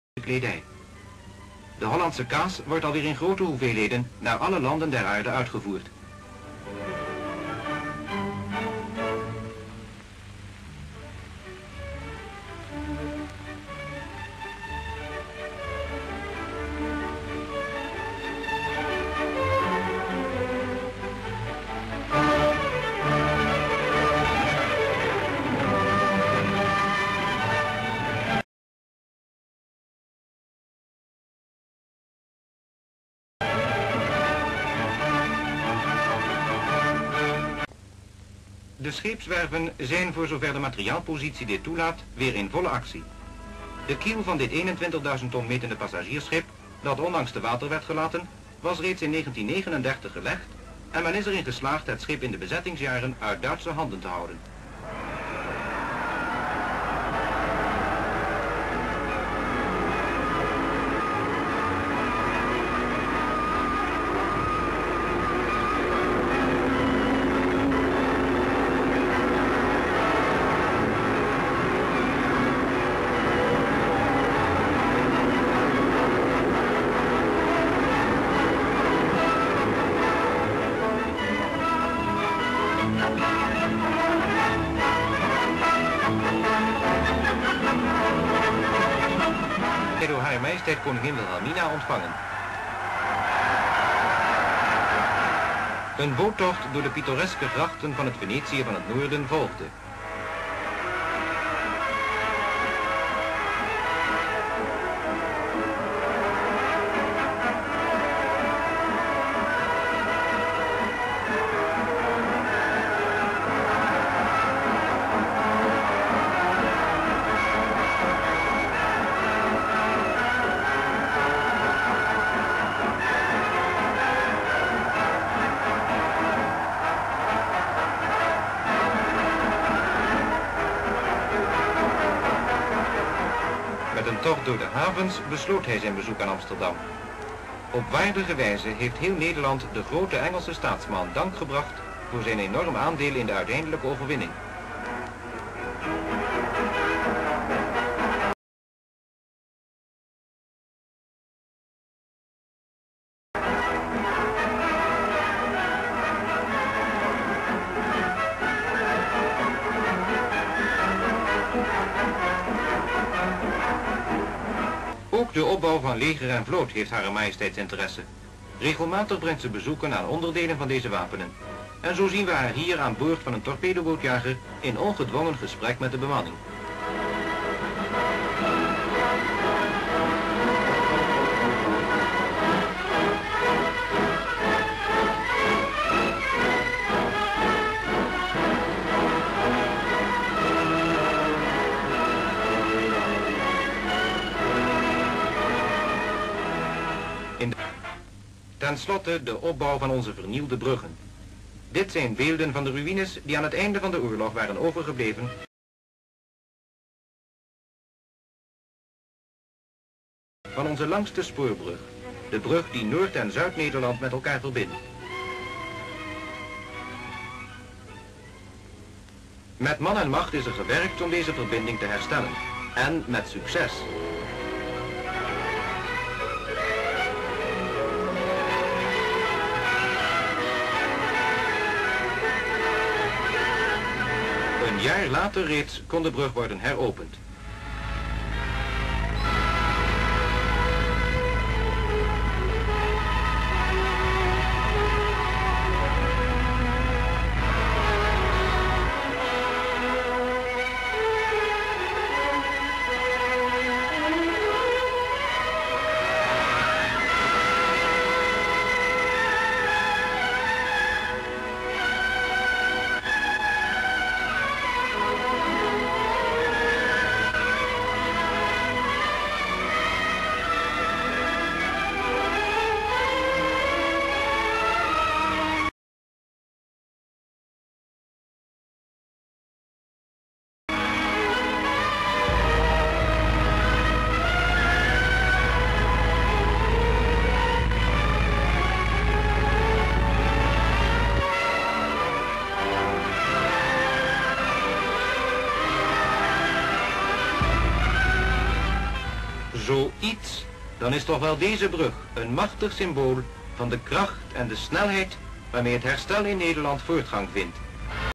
oude schepen 2
Fragmenten met daarin o.a. oude (vracht) schepen net na de tweede wereld oorlog.
Kaas laden in Alkmaar,  passagierschip loopt van stapel, Rondvaart in Amsterdam van Churchill en Juliana, bezoek juliana aan torpedobootjager.
Gerepareerde moerdijkbrug opnieuw in gebruik genomen.